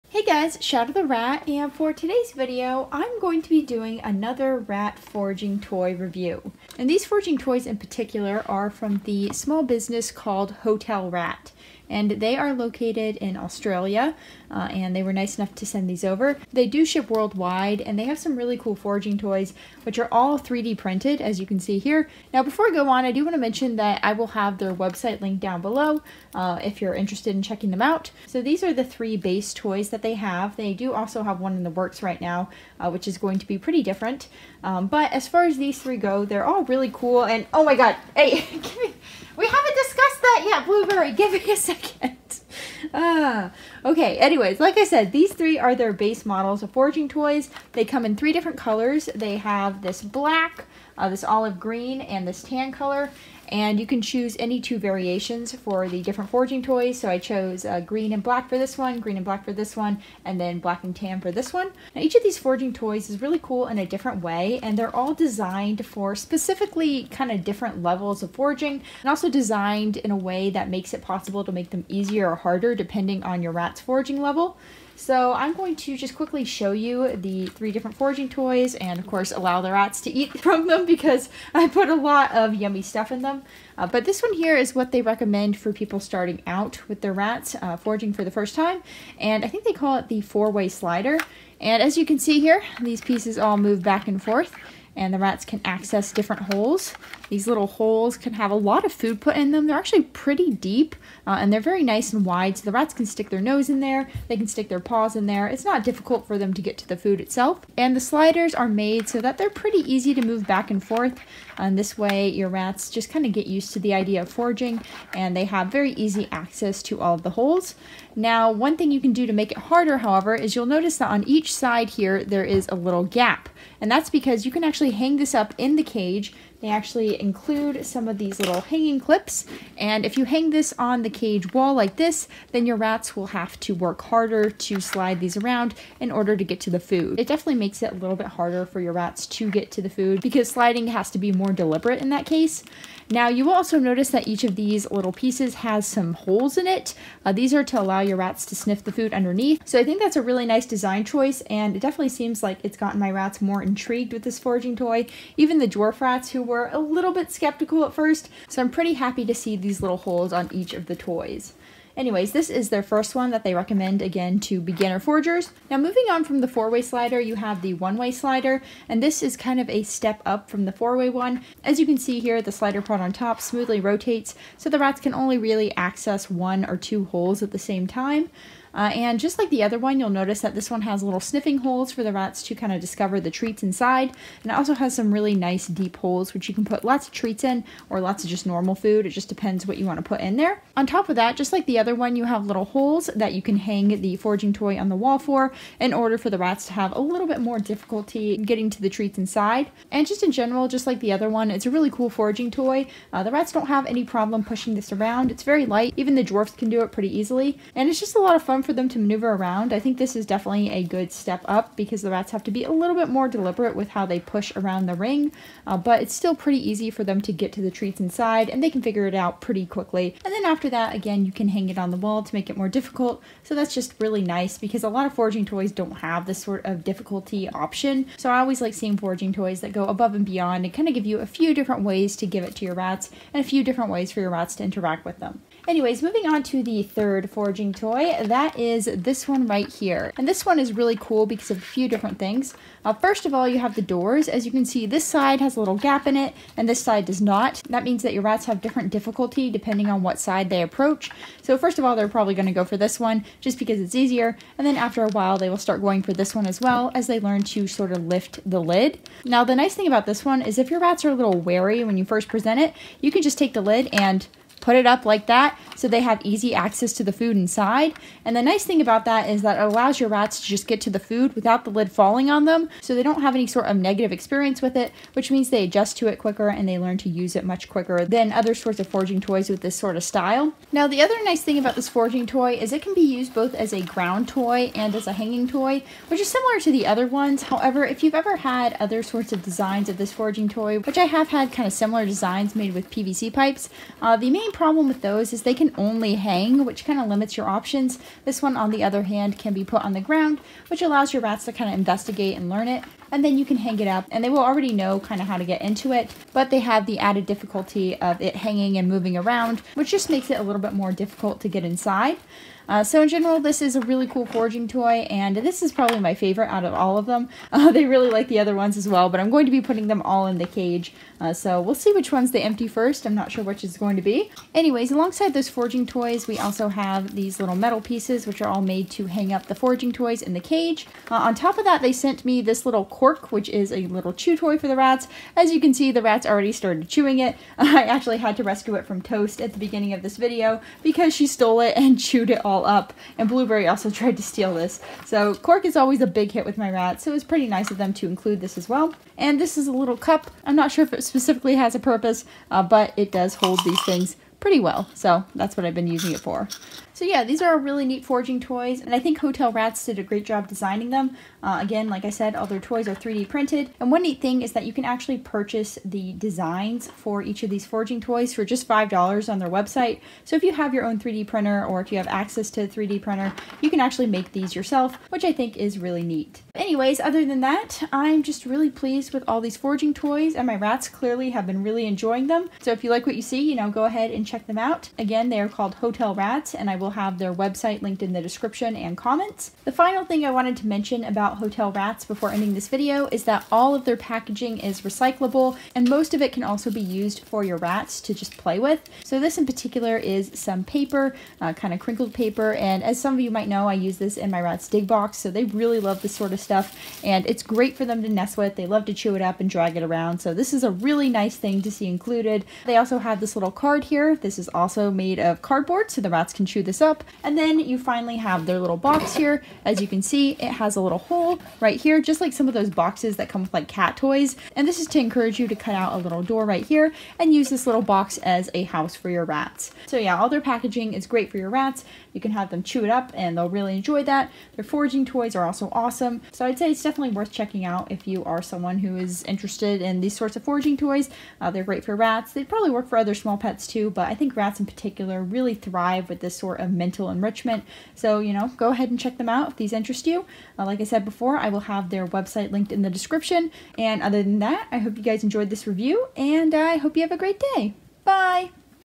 Hotel Rat's AMAZING Foraging Toys (review)!
Today I'll be reviewing some awesome foraging toys by Hotel Rat - you can find these on their